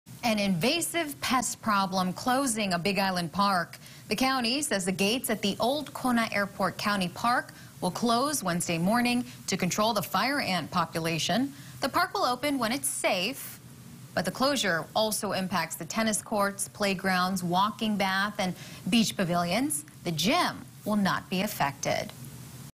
Gates at Old Kona Airport County Park will be closed due to fire ants
The gates at the Old Kona Airport County Park will be closed on Wednesday, Aug. 18, due to a fire ant population.